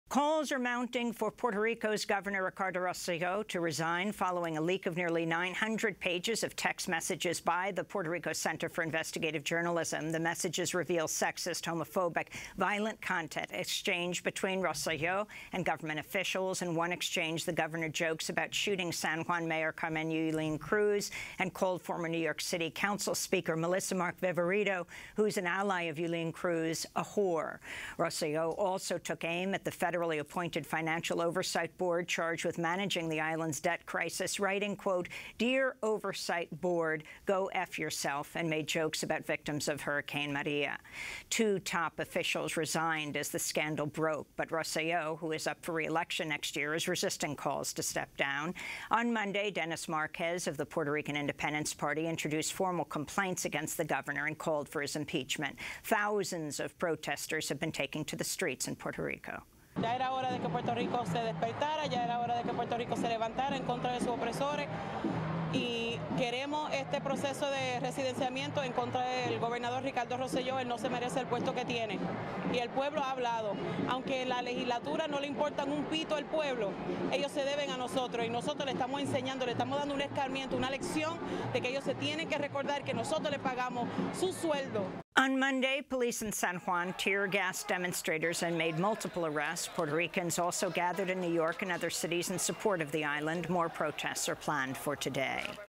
Manifestantes piden la renuncia del gobernador puertorriqueño Rosselló
Vea esta y otras noticias en nuestra página web en español

Siguen aumentando las peticiones para que el gobernador de Puerto Rico, Ricardo Rosselló, renuncie luego de que el Centro de Periodismo Investigativo de Puerto Rico publicara la filtración de casi 900 páginas de mensajes de texto. Los mensajes revelan el intercambio de contenido sexista, homófobo y violento entre Rosselló y autoridades del gobierno.

El lunes, la policía de San Juan roció con gases lacrimógenos a los manifestantes e hizo múltiples arrestos. Los puertorriqueños también se reunieron en Nueva York y otras ciudades en apoyo de la isla.